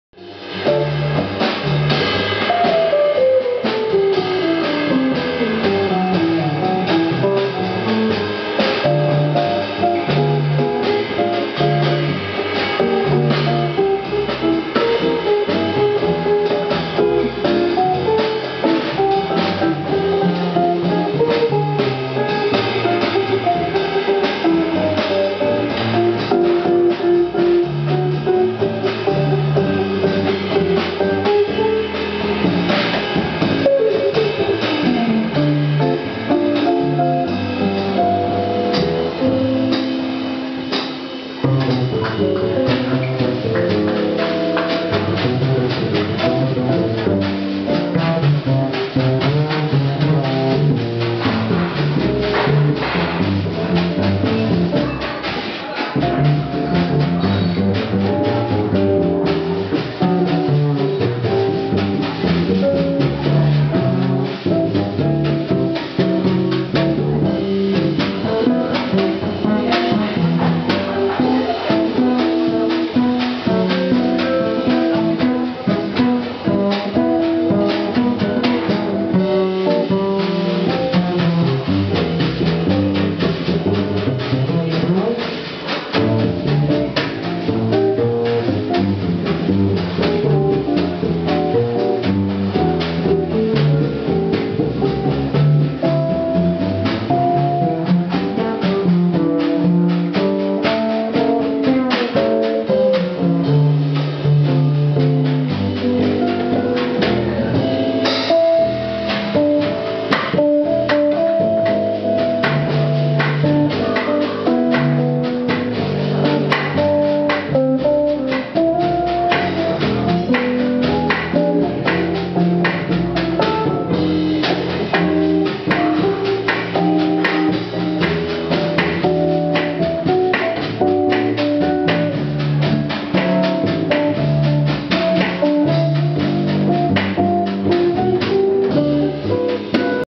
EFM 40th: Jazz at the Blue Cafe, Munich!
Nicolas Delgado Quartet performing at the Blue cafe and bar, Munich Germany